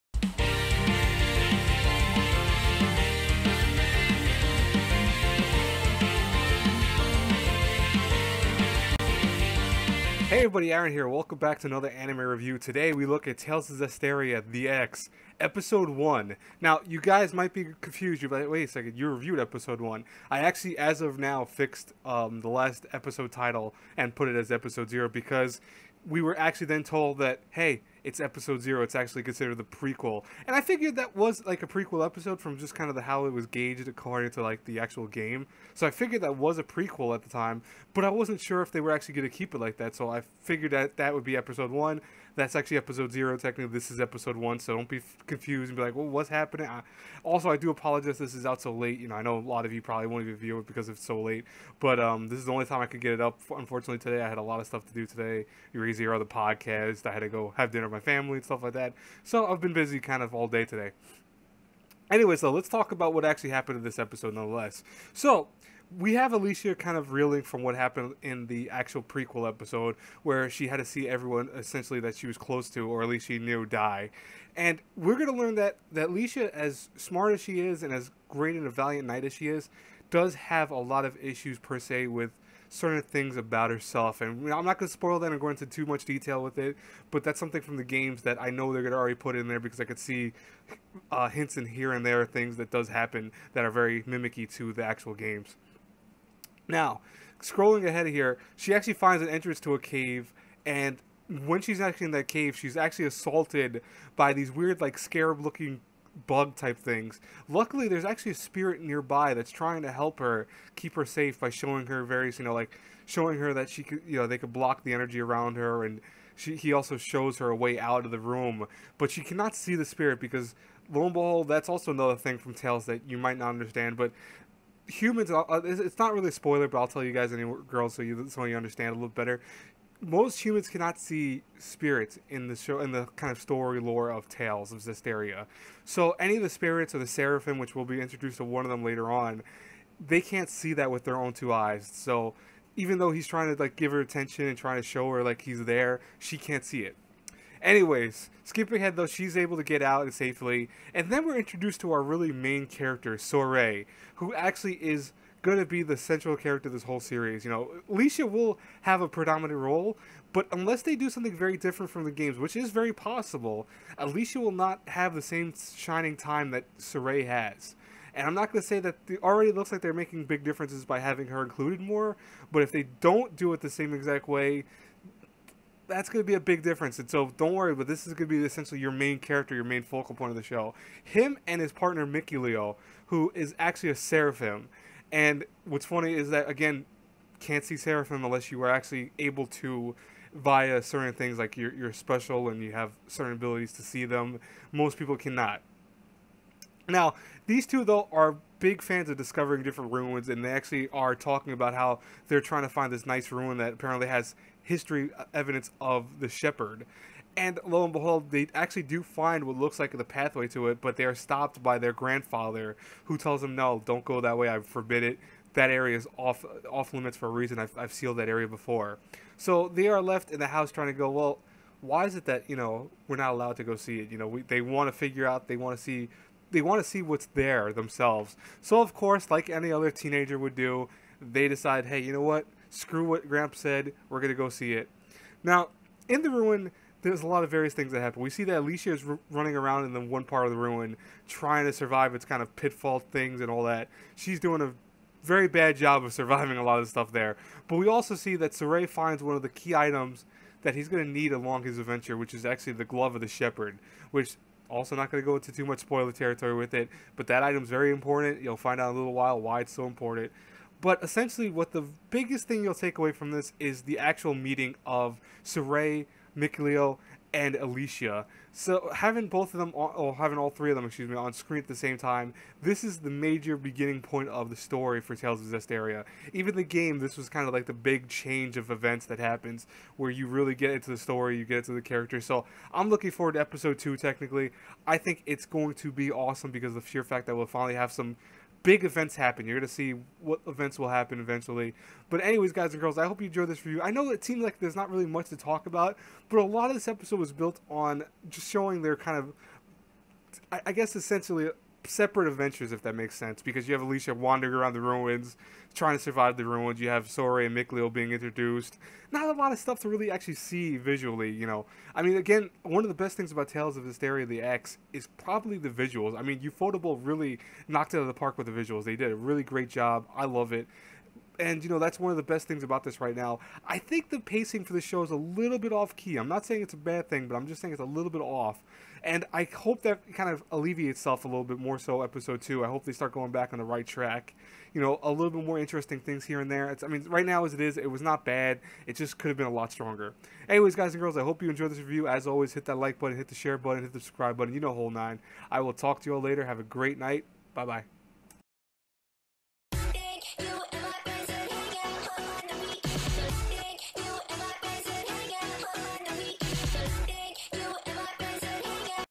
Tales of Zestiria the X テイルズ オブ ゼスティリア ザ クロス Episode 1 Review/Impressions
*Note forgot to put music in the review, sorry folks*
Tales of Zestiria episode 1, cause the other one was actually a prequel, was interesting. We finally are introduced to the central character, Sorey and well as his pal Mikelo. These three will finally cross paths and the true story begins.